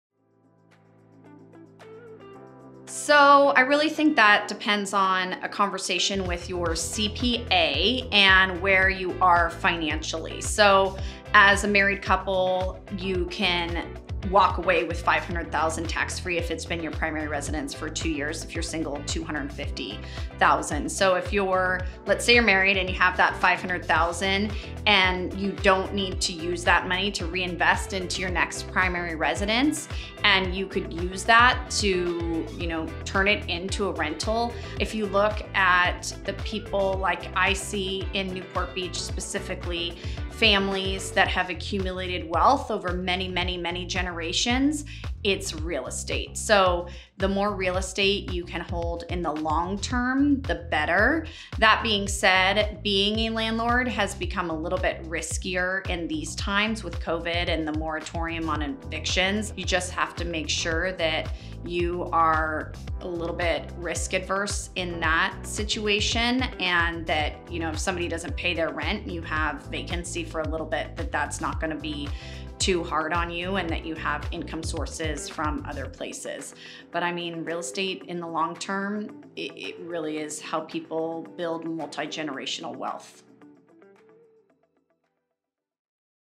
Should I sell my home or use it as passive income in Orange County real estate?
Shoud you sell your home and take the equity or keep it and rent it out and use it as passive income in Orange County, California?